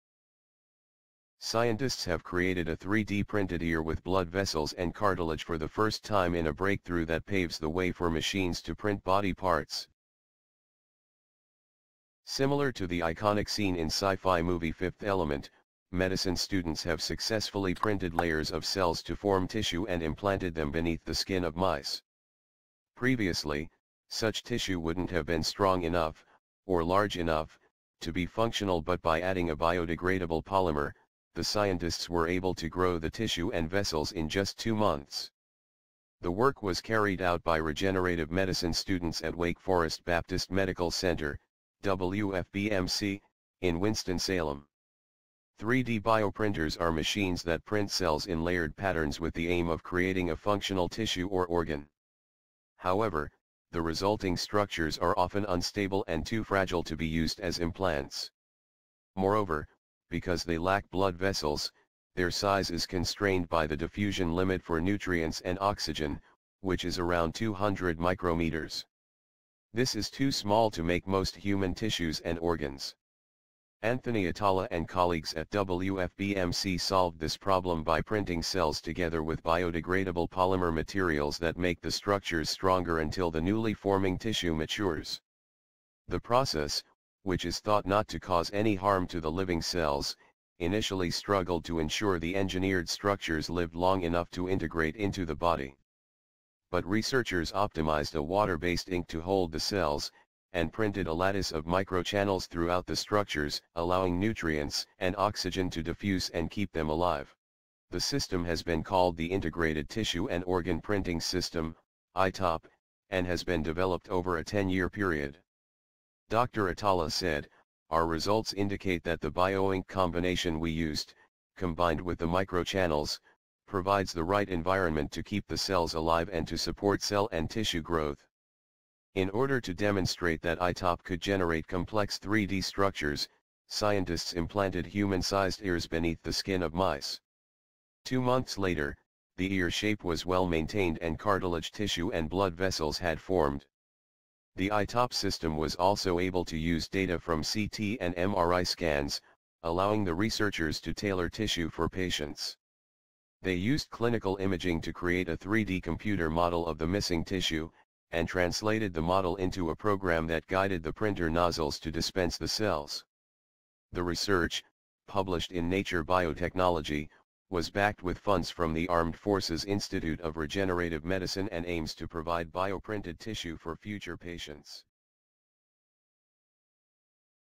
'Fifth Element 3D printer' creates first human ear with blood vessels in breakthrough
'Fifth Element 3D printer' creates first human ear with blood vessels in breakthrough that paves the way for printable body parts
Scientists have created a 3D-printed ear with blood vessels and cartilage for the first time in a breakthrough that paves the way for machines to print body parts.
Similar to the iconic scene in sci-fi movie Fifth Element, medicine students have successfully printed layers of cells to form tissue and implanted them beneath the skin of mice.